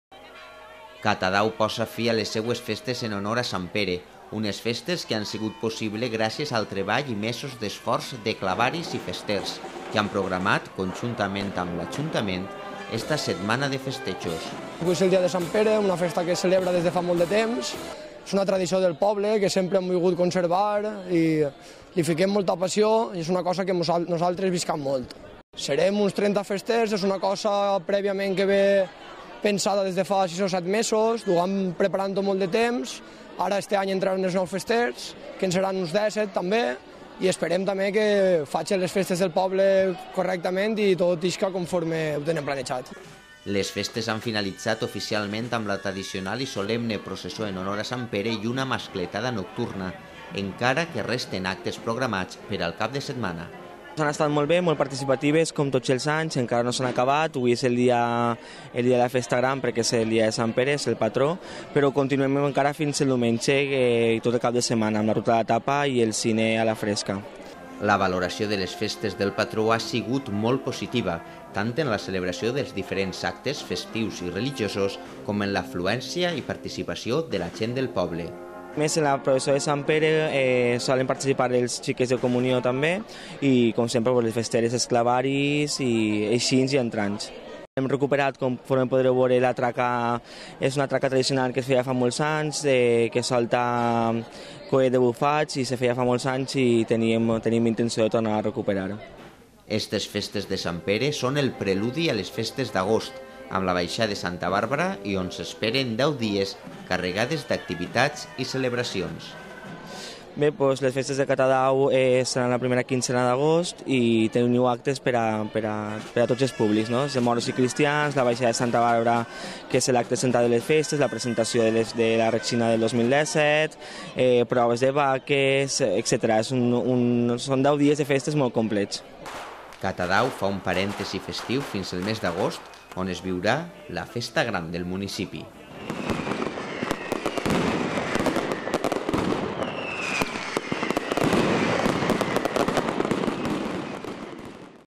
Catadau finalitza les festes en honor al seu patró
La valoració de les festes del patró ha sigut molt positiva, tant en la celebració dels diferents actes com en l’afluència de gent.